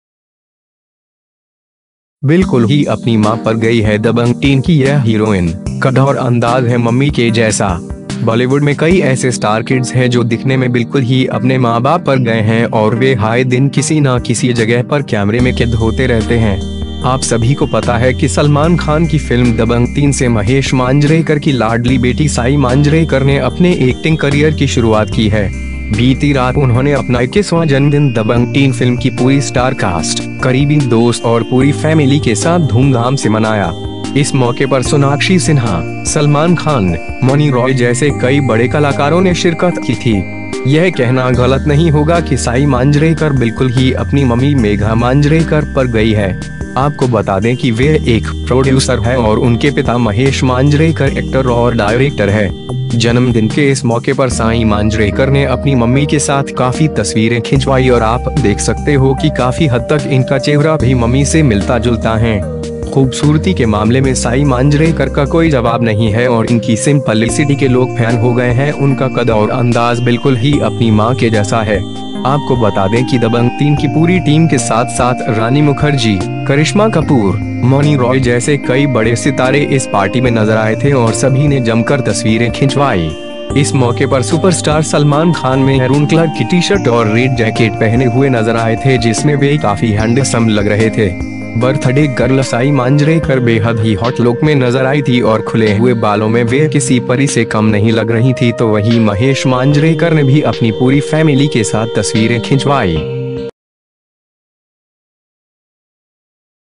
बिल्कुल ही अपनी मां पर गई है 'दबंग 3' की यह हीरोइन, कद और अंदाज है मम्मी के जैसा
बॉलीवुड में कई ऐसे स्टार किड्स है जो दिखने में बिल्कुल ही अपने मां बाप पर गए हैं और वह आए दिन किसी ना किसी जगह पर कैमरे में कैद होते रहते हैं । आप सभी को पता है कि सलमान खान की फिल्म 'दबंग 3' से महेश मांजरेकर की लाडली बेटी साई मांजरेकर ने अपने एक्टिंग करियर की शुरुआत की है । बीती रात उन्होंने अपना 21 वां जन्मदिन दबंग 3 फिल्म की पूरी स्टारकास्ट, करीबी दोस्त और पूरी फैमिली के साथ धूमधाम से मनाया । इस मौके पर सोनाक्षी सिन्हा, सलमान खान, मौनी रॉय जैसे कई बड़े कलाकारों ने शिरकत की थी ।

यह कहना गलत नहीं होगा कि साई मांजरेकर बिल्कुल ही अपनी मम्मी मेघा मांजरेकर पर गई है। आपको बता दें कि वह एक प्रोड्यूसर है और उनके पिता महेश मांजरेकर एक्टर और डायरेक्टर है । जन्मदिन के इस मौके पर साईं मांजरेकर ने अपनी मम्मी के साथ काफी तस्वीरें खिंचवाई और आप देख सकते हो कि काफी हद तक इनका चेहरा भी मम्मी से मिलता जुलता हैं । खूबसूरती के मामले में साई मांजरेकर का कोई जवाब नहीं है और इनकी सिंपलीसिटी के लोग फैन हो गए हैं उनका कद और अंदाज बिल्कुल ही अपनी मां के जैसा है ।

आपको बता दें कि दबंग 3 की पूरी टीम के साथ साथ रानी मुखर्जी, करिश्मा कपूर, मौनी रॉय जैसे कई बड़े सितारे इस पार्टी में नजर आए थे और सभी ने जमकर तस्वीरें खिंचवाई । इस मौके पर सुपरस्टार सलमान खान मेहरून कलर की टी-शर्ट और रेड जैकेट पहने हुए नजर आए थे जिसमें वह काफी हैंडसम लग रहे थे ।

बर्थडे गर्ल साई मांजरेकर बेहद ही हॉट लुक में नजर आई थी और खुले हुए बालों में वह किसी परी से कम नहीं लग रही थी तो वही महेश मांजरेकर ने भी अपनी पूरी फैमिली के साथ तस्वीरें खिंचवाई ।